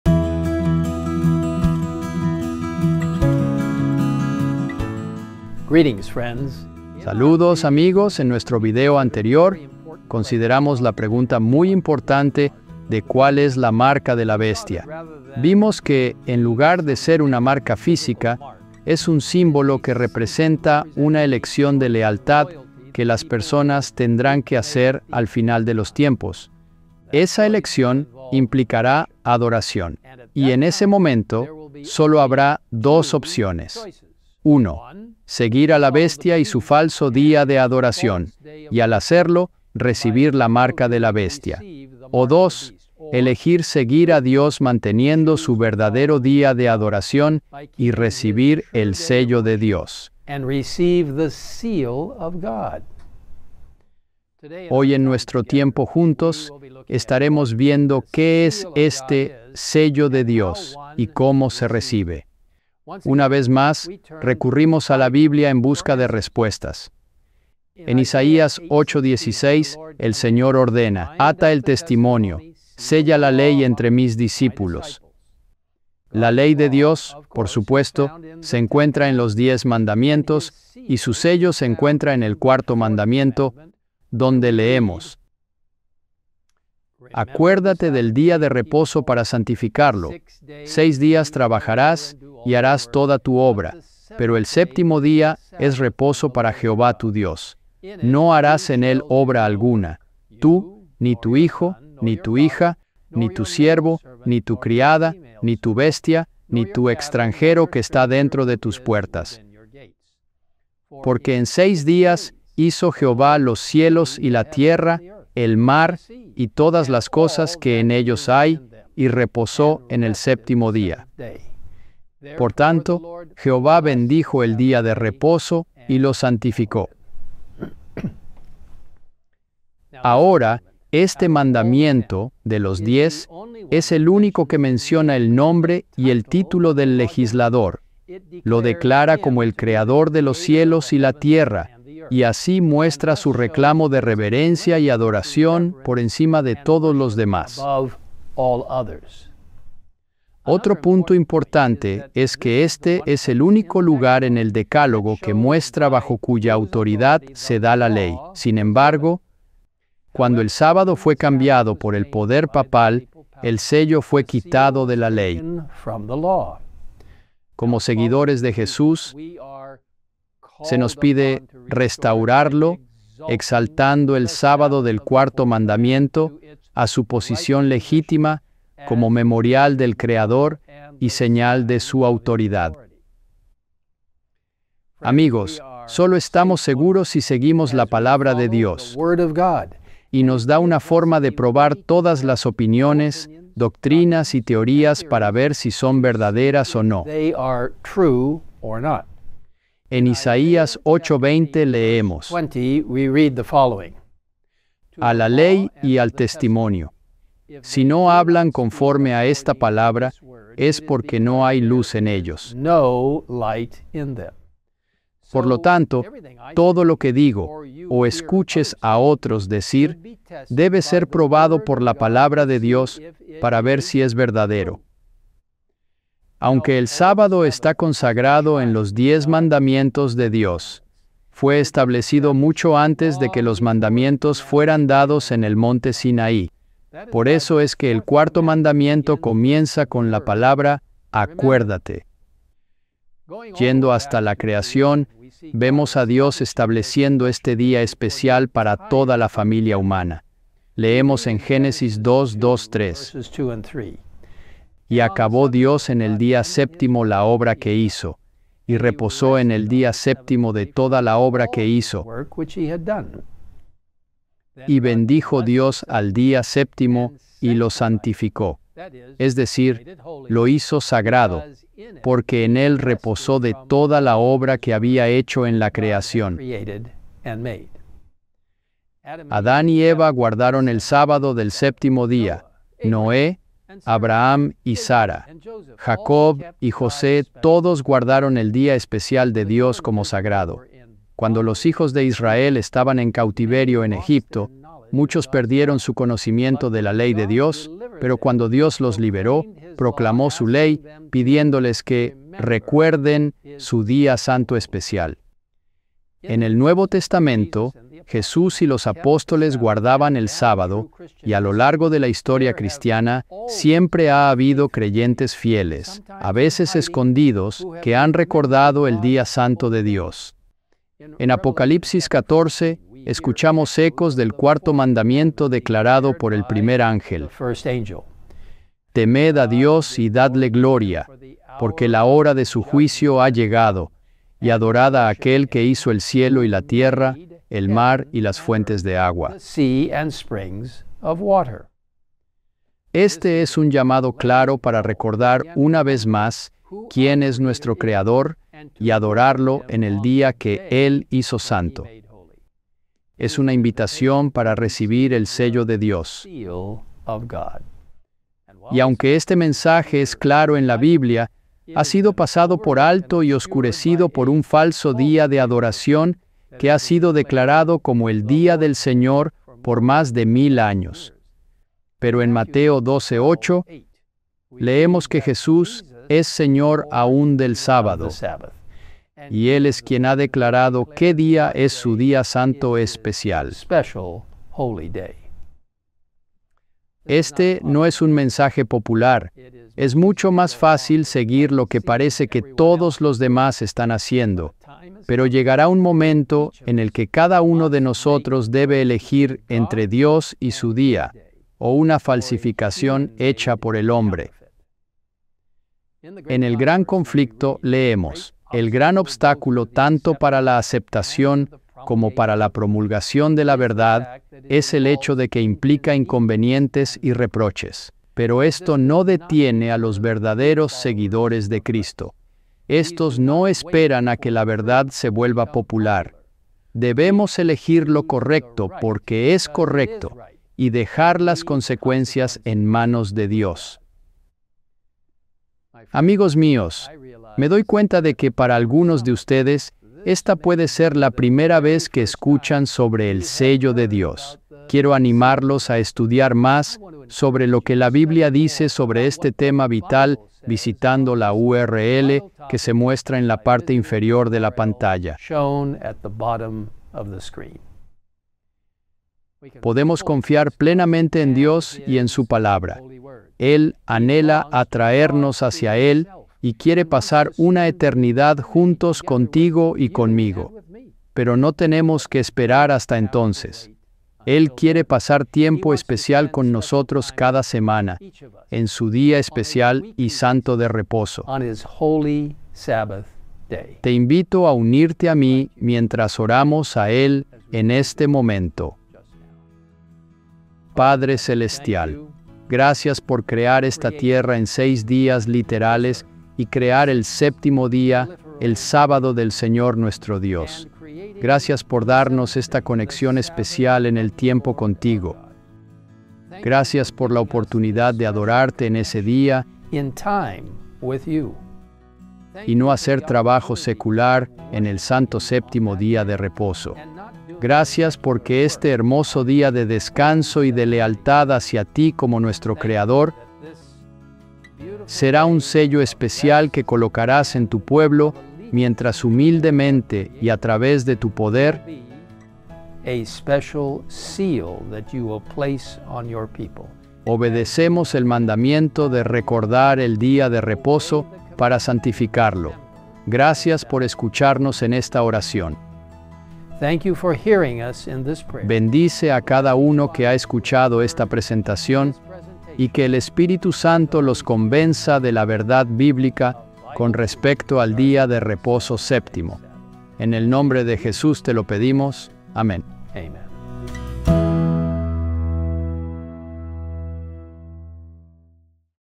La Gran Controversia: El Sello de Dios | Pastor Ted Wilson
Únete al Pastor Ted Wilson, presidente de la Iglesia Adventista del Séptimo Día, mientras continúa adentrándose en El Gran Conflicto de Ellen White, esta vez, discutiendo el sello de Dios.

¿Cuál es el Sello de Dios? ¿Cómo podemos obtenerlo? Escucha toda la discusión para obtener respuestas.

🔗 Para aprender más sobre el Sello de Dios y su importancia en tu vida, lee El Gran Conflicto de Ellen White y únete a nosotros la próxima semana para más.